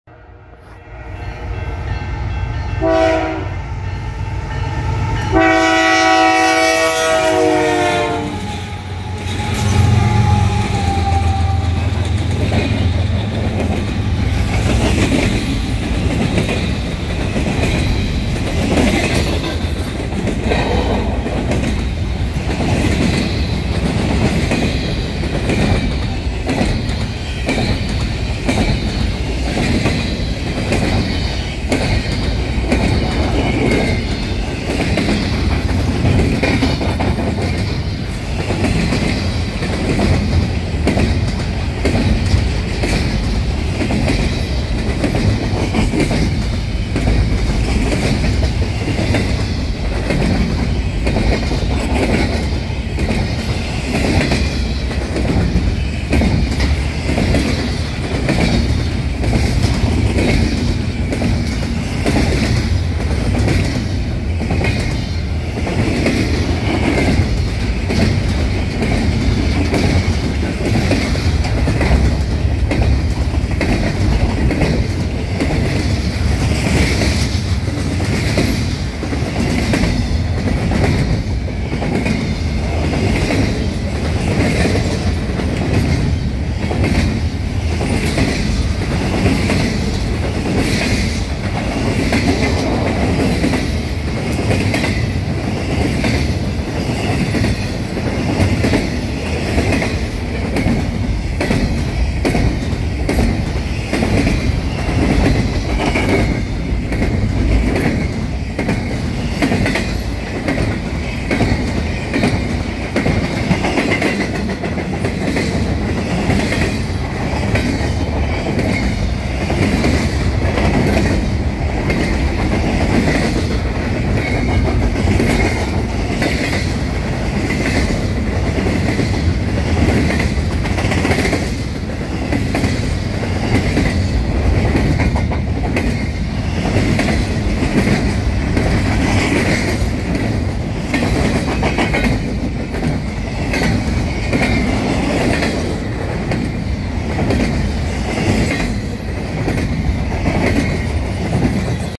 CPKC Double Stack Train Rolling Through Glen Valley British Columbia Canada
Filmed at Bradner Road crossing Glen Valley British Columbia Canada CN Yale Sub

Train : CPKC A71 West

Power :
CPKC 9766 GE AC4400CW
CPKC 9704 GE AC4400CW
CPKC 8827 GE ES44AC GEVO